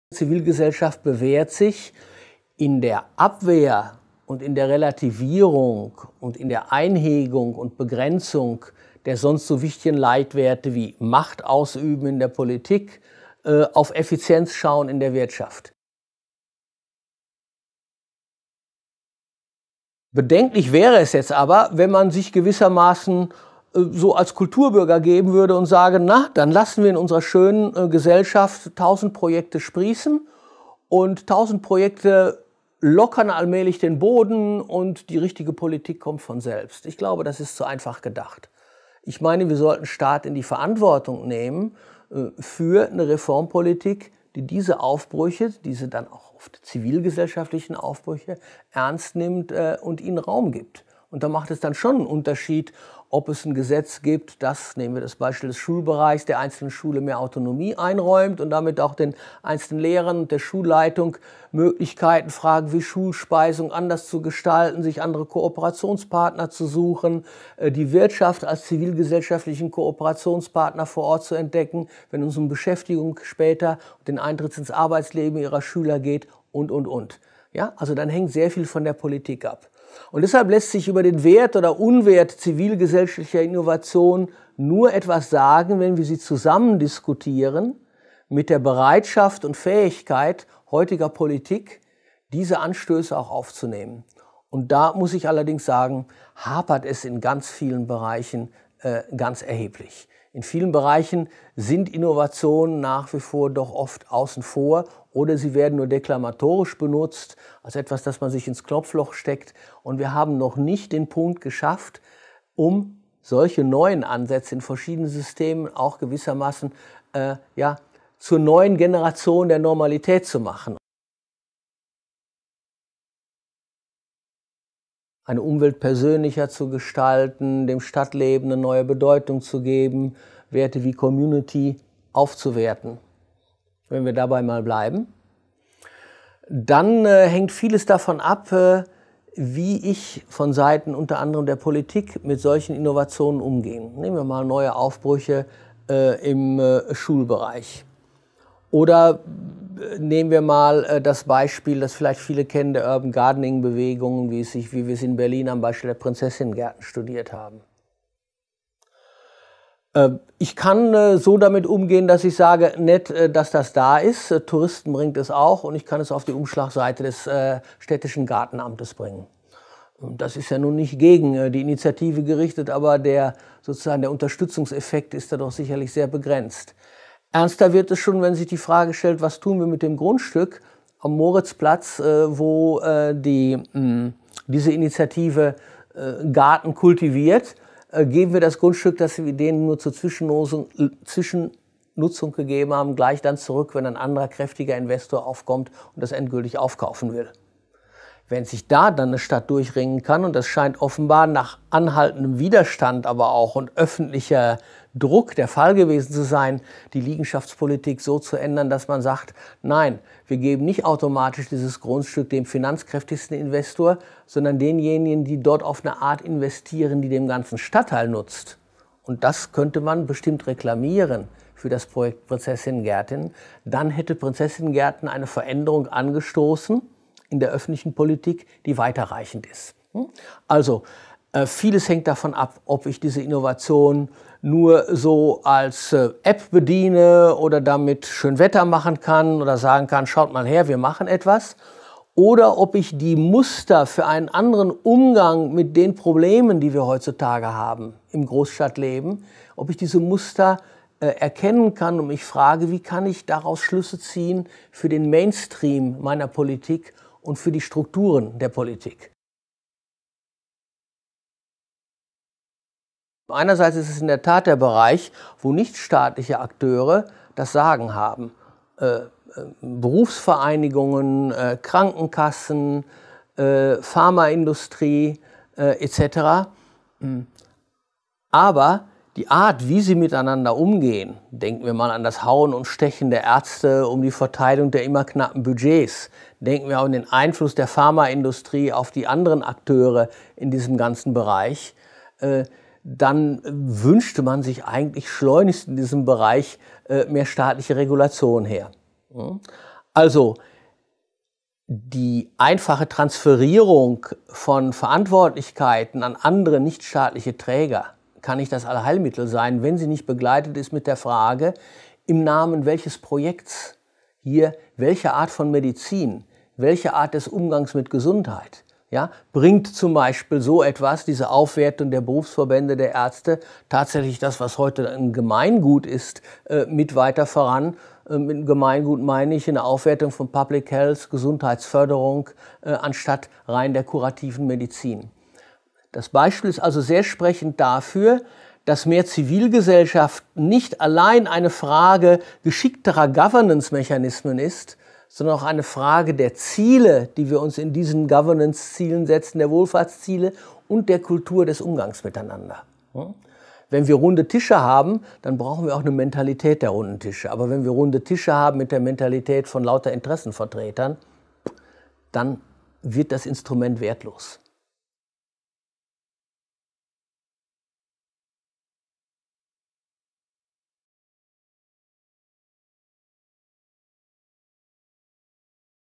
Adalbert Evers: Bürger, mehr Selbstbewusstsein!
Wie können Impulse aus der Zivilgesellschaft zu dauerhafter Innovation führen, wo liegen die Grenzen? Adalbert Evers ist Professor für Vergleichende Gesundheits- und Sozialpolitik an der Universität Gießen.